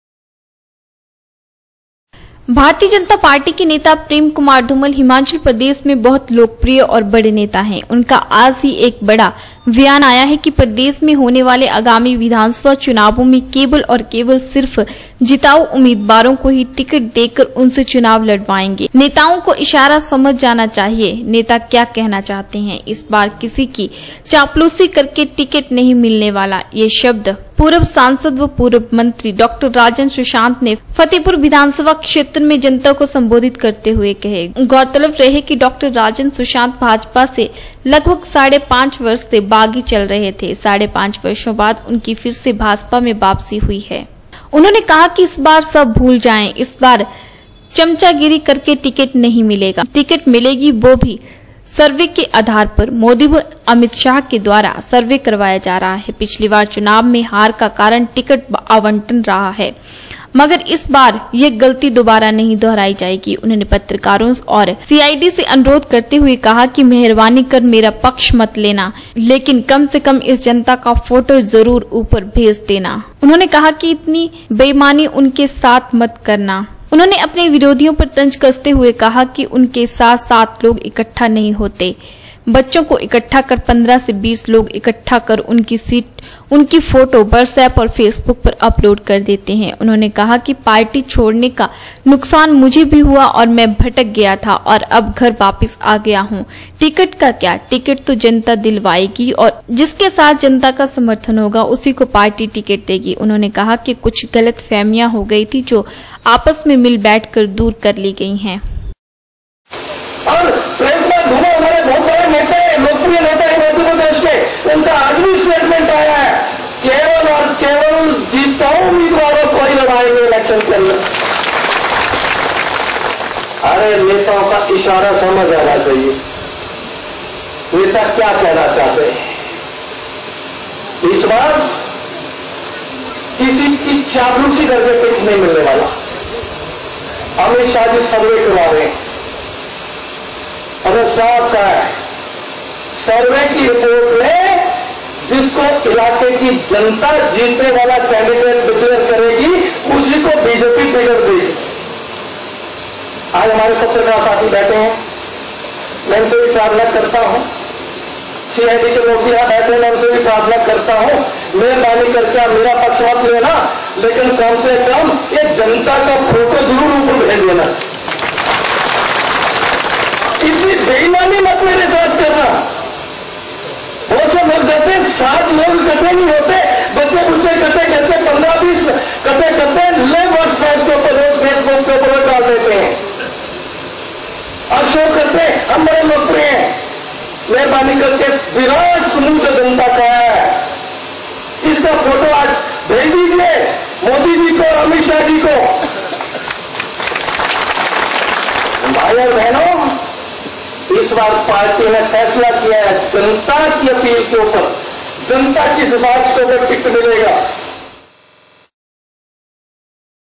Rajan Sushant on BJP tickets
प्रदेश में होने वाले आगामी विधानसभा चुनाव में केवल और केवल सिर्फ जिताऊ उम्मीदवारों को ही टिकेट देकर उनसे चुनाव लड़वायेंगेएपार्टी के नेताओं का इशारा समझ जाना चाहिए की वह क्या कहना चाहते हैं इस बार किसी की चापलूसी करके टिकेट नहीं मिलने वाला यह शब्द पूर्व सांसद एवं पूर्व मंत्री डॉ राजन सुशांत ने फतेहपुर कहे    उन्होंने कहा कि इस बार चमचागीरी करके टिकट नही मिलेगी । टिकट मिलगी केवल उसी को मिलेगी जो मोदी व अमित शाह के द्धारा करवाए गए सर्वे करबाया में सही पाए जाएगें । उन्हाने कहा कि  पिछली बार चुनाव मे हार का कारण टिकट आबंटन रहा है मगर इस बार यह गल्ती दौबारा नही दोहराई जाएगी